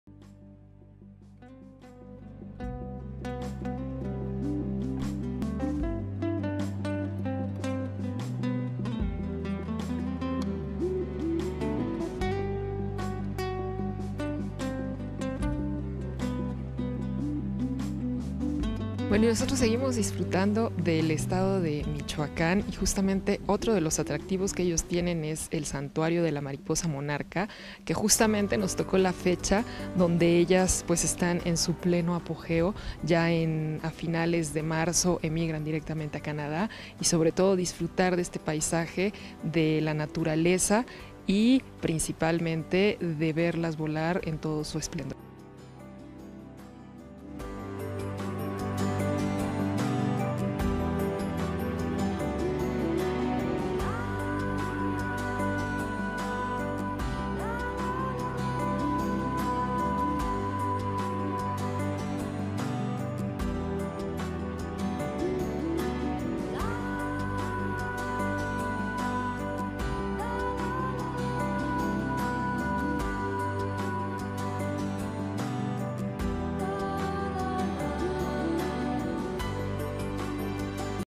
MONARCH BUTTERFLY · BIOSPHERE RESERVE
The Monarch Butterfly Biosphere Reserve (Spanish: Reserva de Biosfera de la Mariposa Monarca) is a World Heritage Site containing most of the over-wintering sites of the eastern population of the monarch butterfly. The reserve is located in the Trans-Mexican Volcanic Belt pine-oak forests ecoregion on the border of Michoacán and State of Mexico, 100 km (62 miles), northwest of Mexico City. Millions of butterflies arrive in the reserve annually. Butterflies only inhabit a fraction of the 56,000 hectares of the reserve from October–March. The biosphere’s mission is to protect the butterfly species and its habitat.
The annual migration cycle of the monarch butterfly has been described as the most spectacular in the insect world, an “endangered natural phenomenon”. This species and its migration are dependent upon conservation of habitats in all three North American countries: Canada, the United States, and Mexico.

MIX: THEBOSS · DCHIC TV